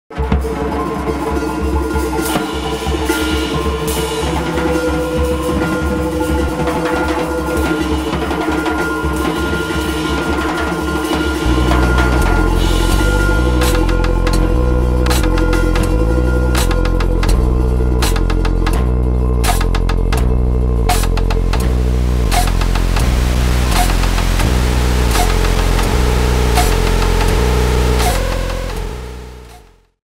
Jazzfestival Willisau 2013
Gehrigtrick & Sohn wünscht allerseits ein amöbiges Jazzfestival Willisau 2013!
Gehrigtrick & Sohn wishes a amoebic Jazzfestival Willisau 2013 to everyone!

Grafik: Annik und Paula Troxler
Animation: Maja Gehrig
Sound: Julian Sartorius